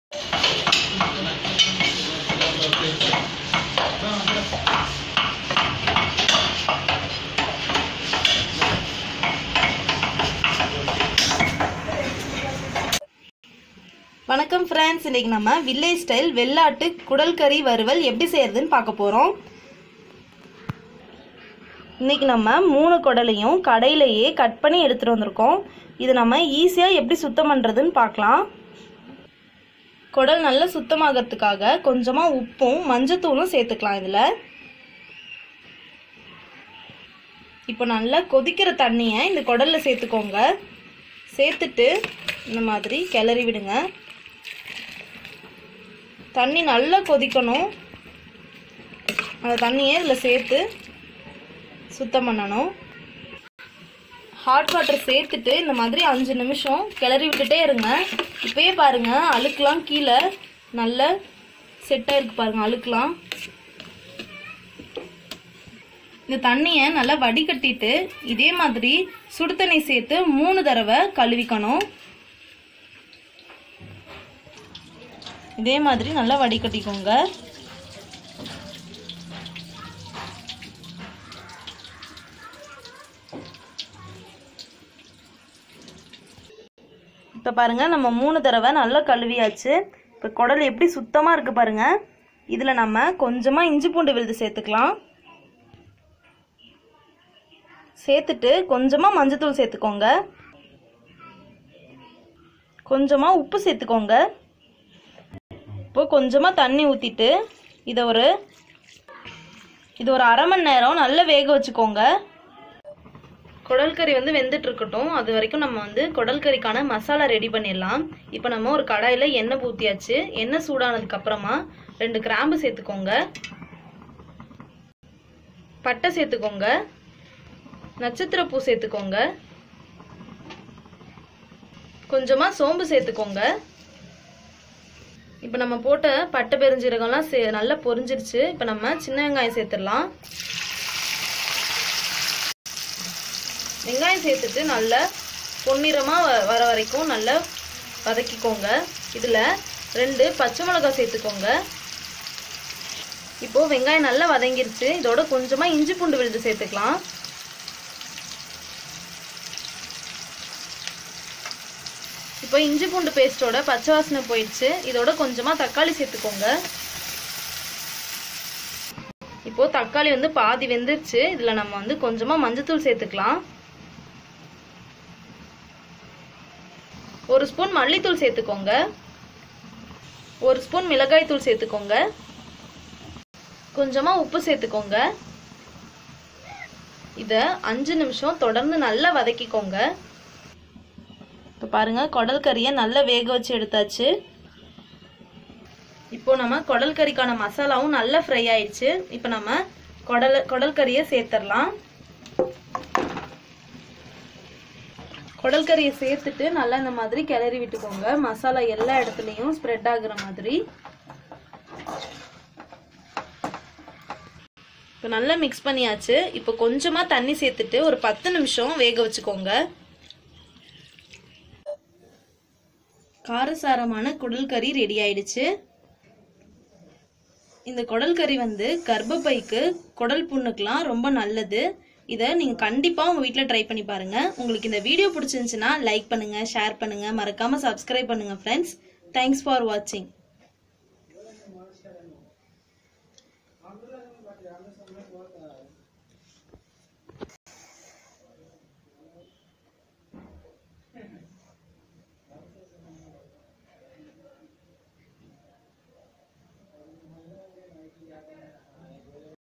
kudal varuval in tamil | குடல்கறி வறுவல் | boti varuval
In this video we are showing how to make kudal varuval.It is rich in nutrients and proteins.It also contains omega 3 fatty acids. It has anti-inflammatory properties and is good for your heart.
Hope you to try this recipes.

# TRY IT TASTE AND LET ME KNOW #